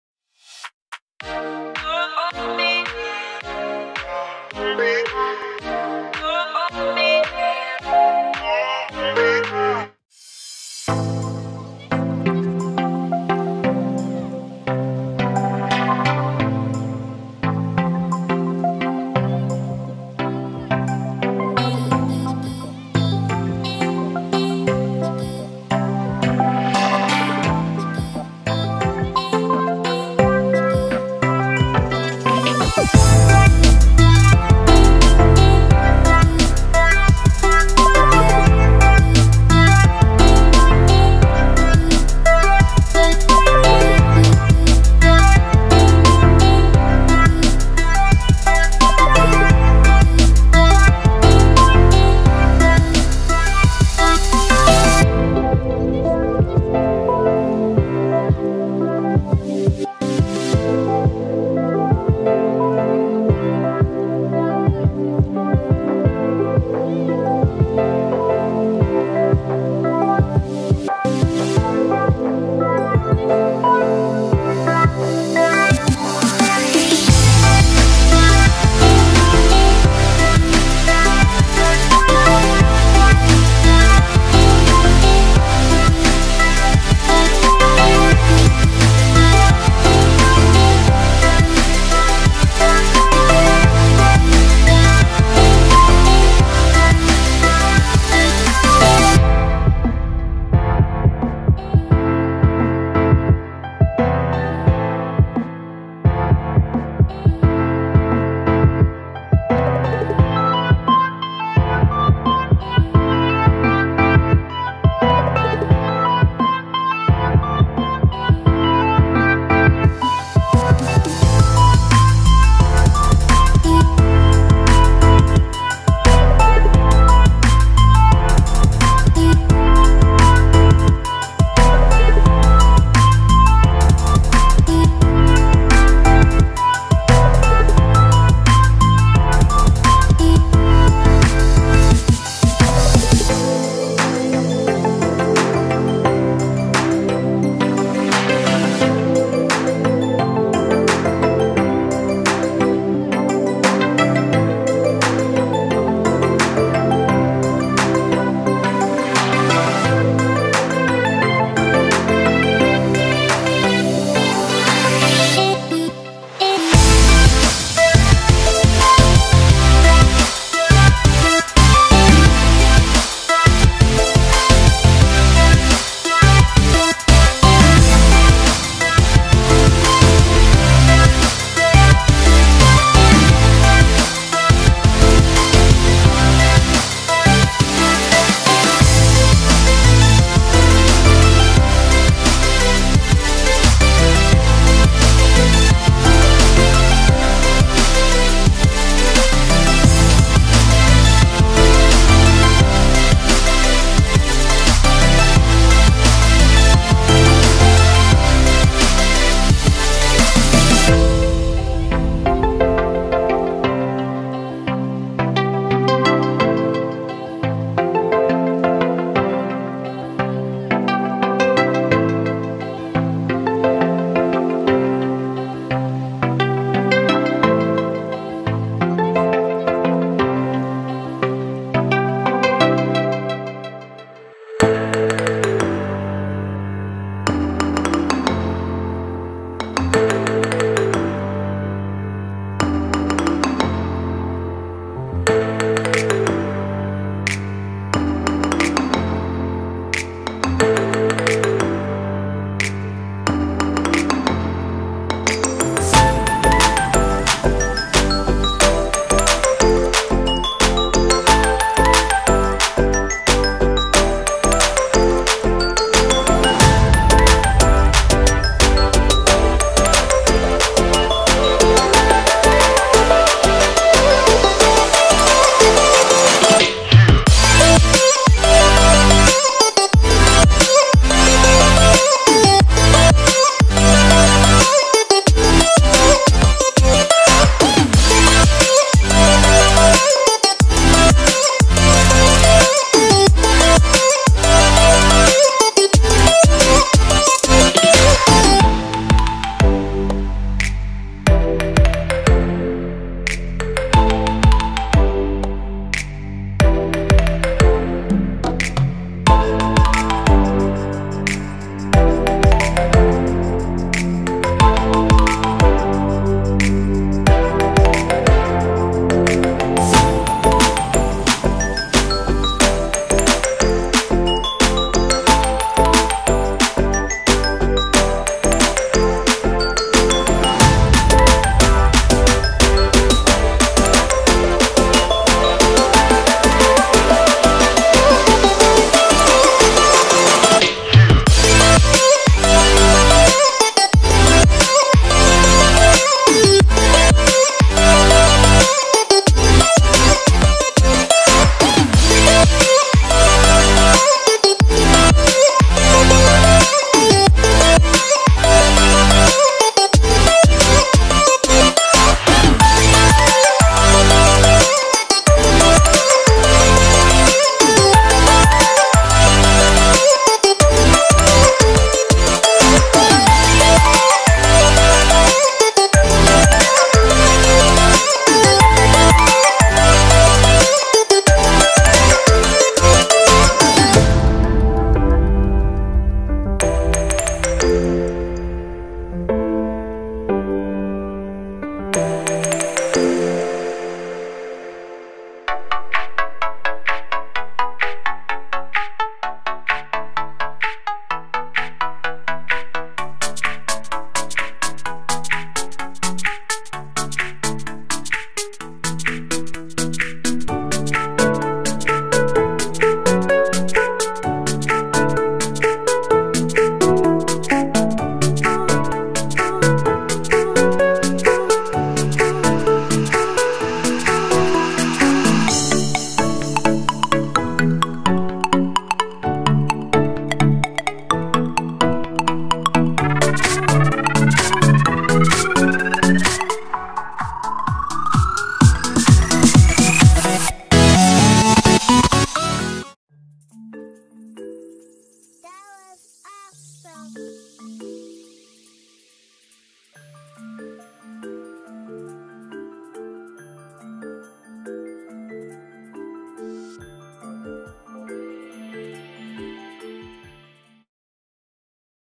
DIY Posable OOAK Ewok Art Doll [Part 3 - Paint & wire Armature]
Here is how I made 3 posable Ewok art dolls, watch all 4 videos to see the process from start to finish.